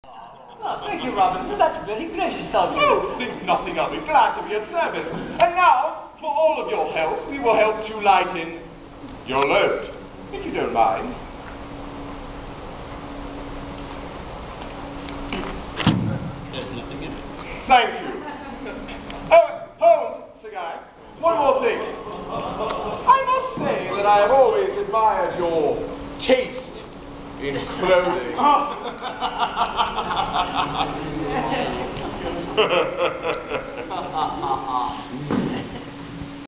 Robin Hood and Guy of Gisbourne
Rehearsals about a week before opening.